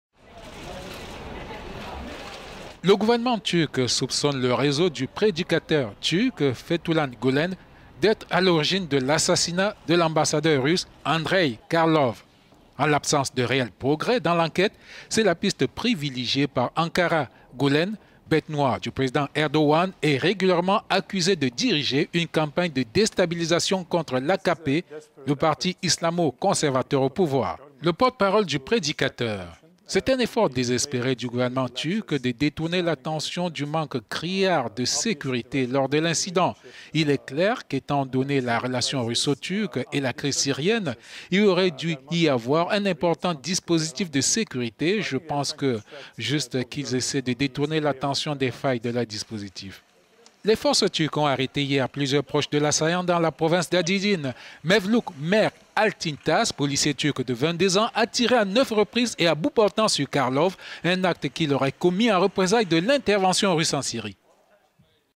Turquie - attentat : Ankara vise Fethullah Gülen
Le gouvernement turc soupçonne le réseau du prédicateur turc Fethullah Gülen d'être à l'origine de l'assassinat de l'ambassadeur russe Andreï Karlov.

En l'absence de réels progrès dans l'enquête, c'est la piste privilégiée par Ankara. Gülen, bête noire du Président Erdogan, est régulièrement accusé de diriger une campagne de déstabilisation contre l'AKP, le parti islamo-conservateur au pouvoir.

"C'est un effort désespéré du gouvernement turc de détourner l'attention du manque criant de séc…